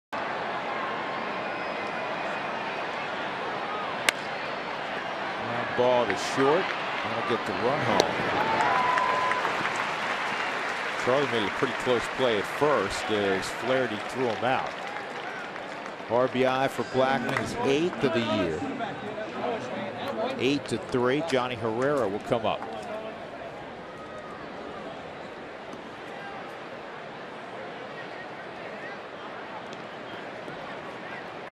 2013/08/17 Arenado scores on groundout
COL@BAL: Arenado races home to score on a groundout